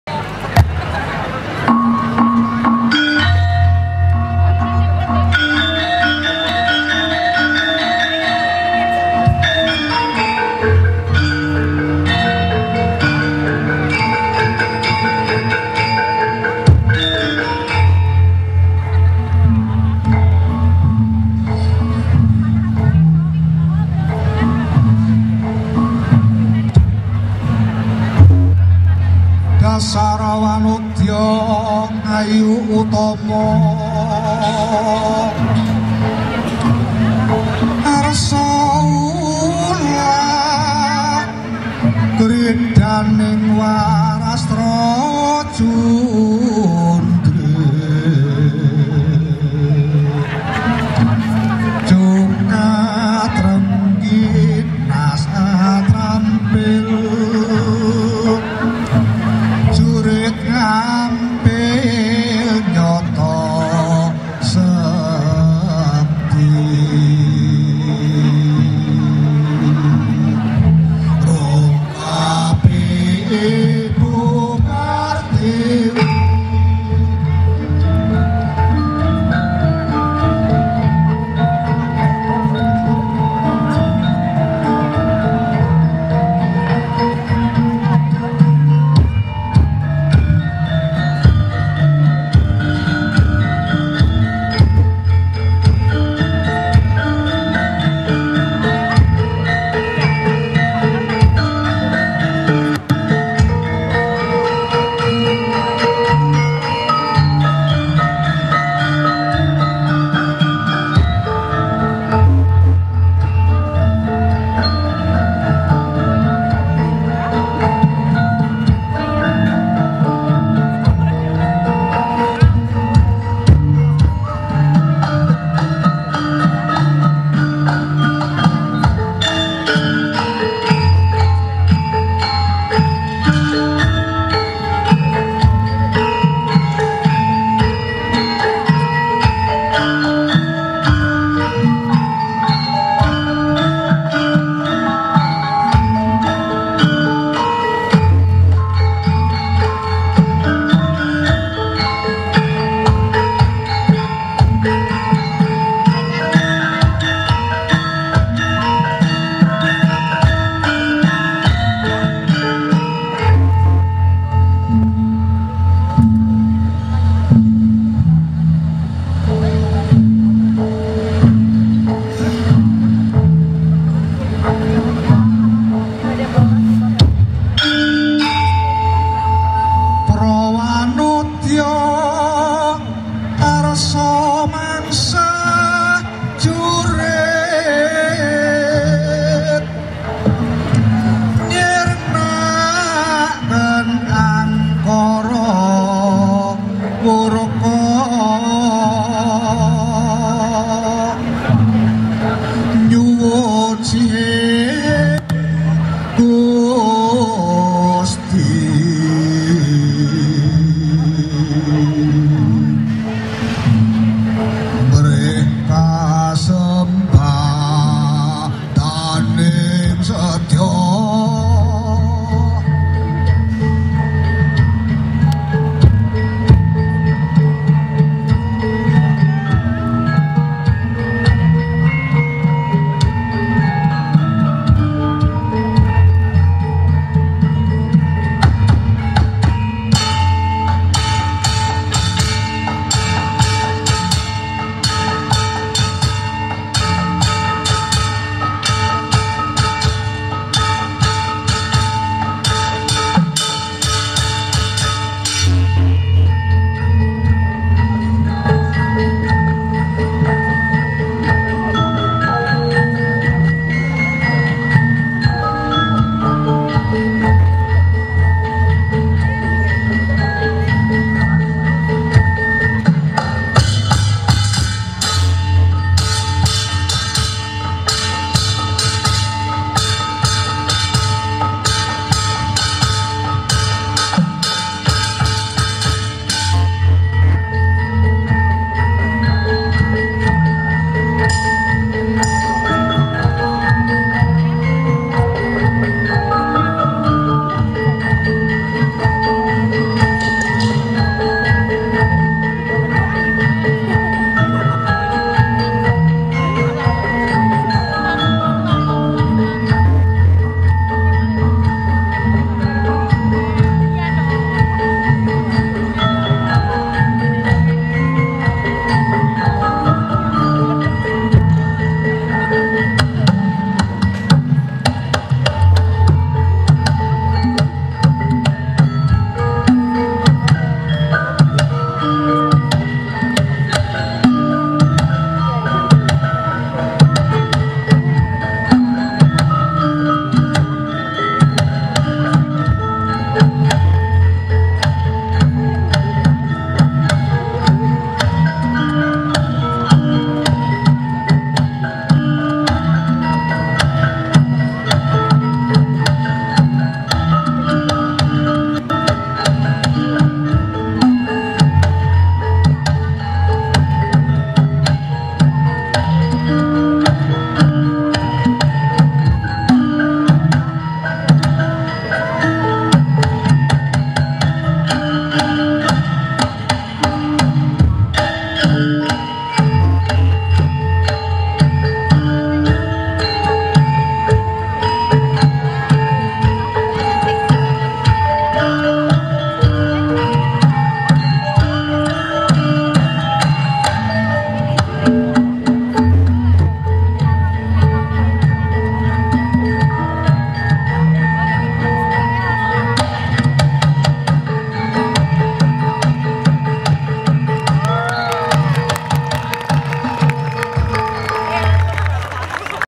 Tari Jurit Ampil dalam acara Pergelangan Budaya Indonesia di UKSW Salatiga 2022
Sanggar Tari Hasthasawanda Kota Salatiga